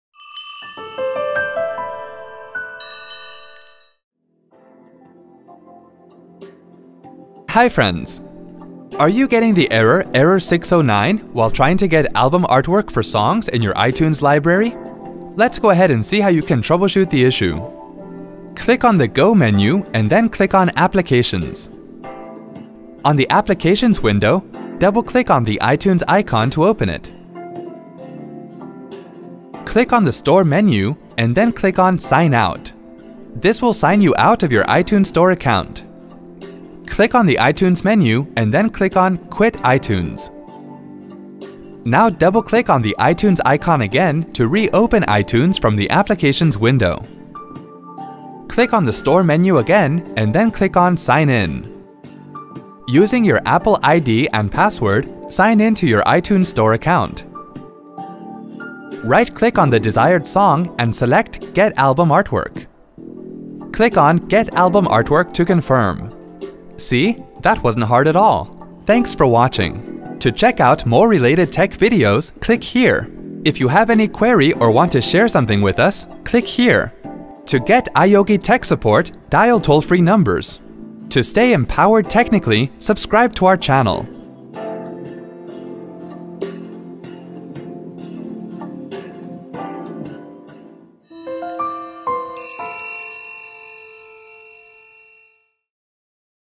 How to fix “Error 609” in iTunes® :Tutorial
Are you getting the error “Error 609” while trying to get album artwork for songs in your iTunes® Library? If you wish to fix the issue when you are getting the error “Error 609” in iTunes® then follow the easy steps shown in this video.